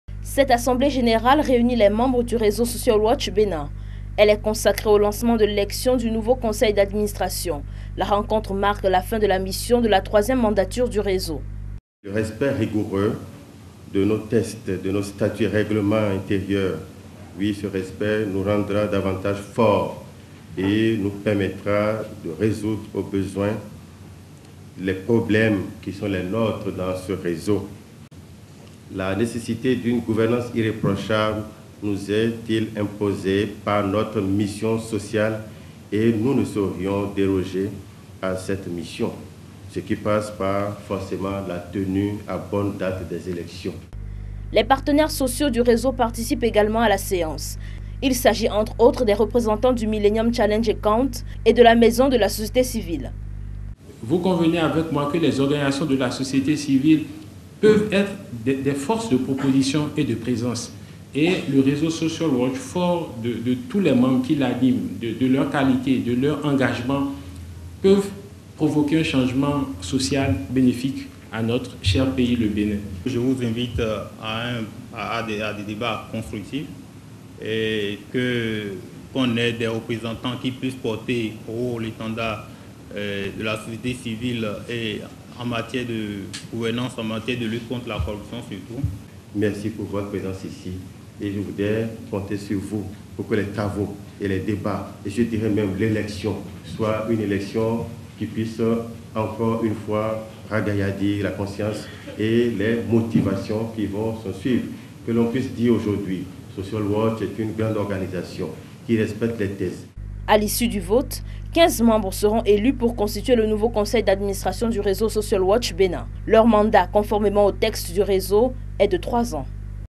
Assemblée Génrale élective du Réseau Social Watch Bénin
Suivez le reportage de Canal 3 Bénin sur la brève cérémonie de lancement des activités de l'Assemblée Générale élective du réseau Social Watch Bénin.